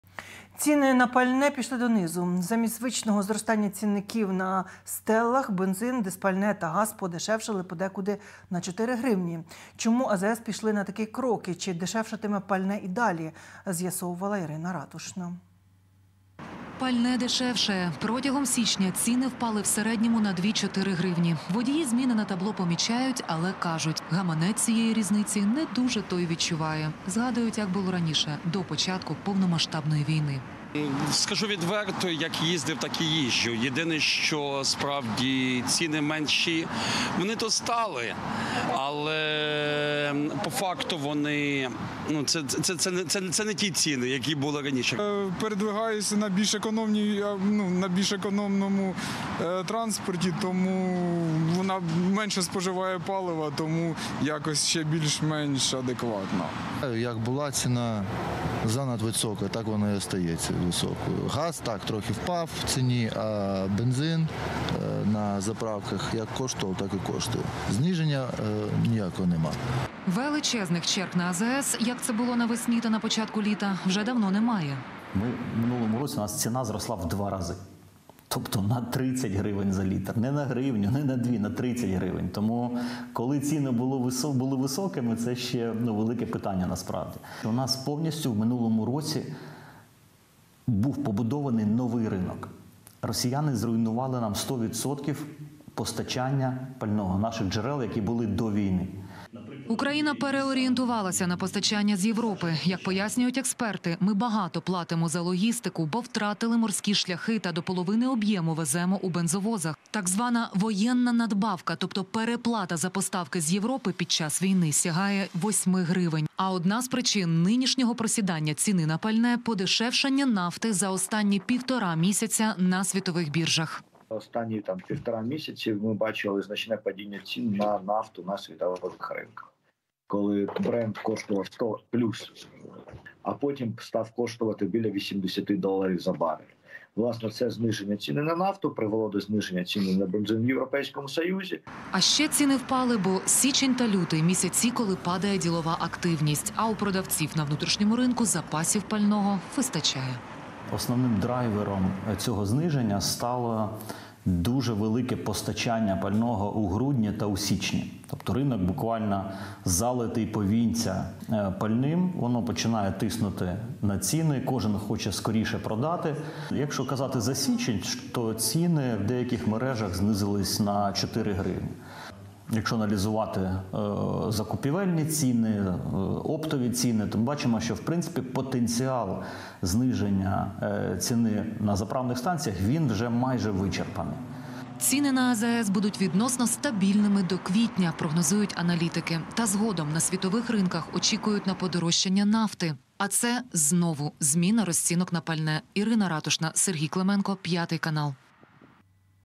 ⛽️⚡️Ціни на пальне пішли вниз: причини і що буде далі
Ціни на пальне пішли донизу. Замість звичного зростання цінників на стелах бензин, дизпальне та газ подешевшали подекуди на 4 гривні. Чому АЗС пішли на такий крок? І чи дешевшатиме пальне й далі, з'ясовувала Ірина Ратушна.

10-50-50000 - будь-яка сума має значення.
Разом переможемо!
Слава Україні!